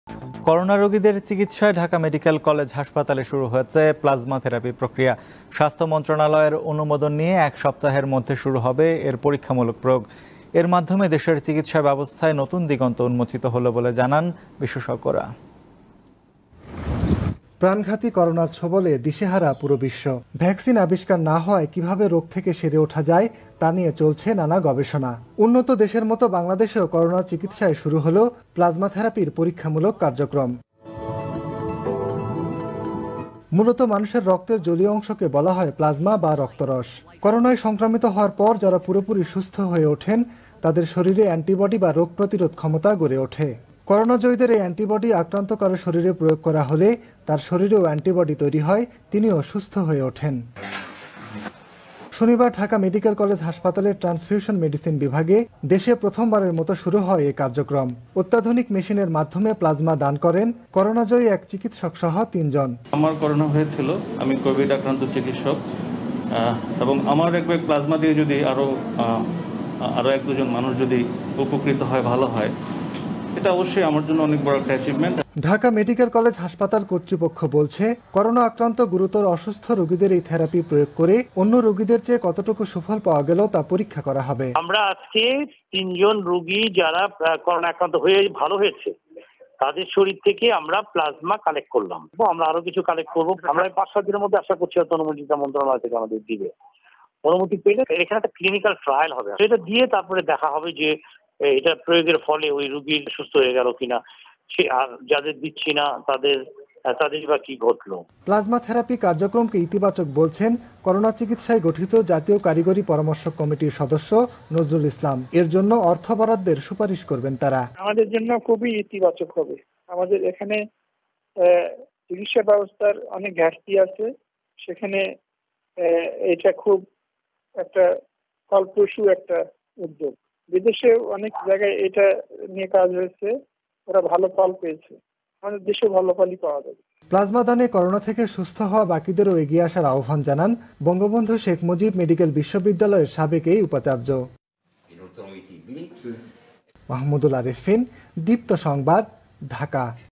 ঢামেকে পরীক্ষামূলক প্লাজমা থেরাপি শুরু
করোনারোগীদের চিকিৎসায় ঢাকা মেডিকেল কলেজ হাসপাতালে শুরু হয়েছে প্লাজমা থেরাপি প্রক্রিয়া। স্বাস্থ্য মন্ত্রণালয়ের অনুমোদন নিয়ে এক সপ্তাহের মধ্যে শুরু হবে এর পরীক্ষামূলক প্রয়োগ। এর মাধ্যমে দেশের চিকিৎসা ব্যবস্থায় নতুন দিগন্ত উন্মোচিত হলো বলে জানান বিশেষজ্ঞরা।

Deepto TV address:
7/A/GA Tejgaon Industrial Area, Dhaka 1208, BANGLADESH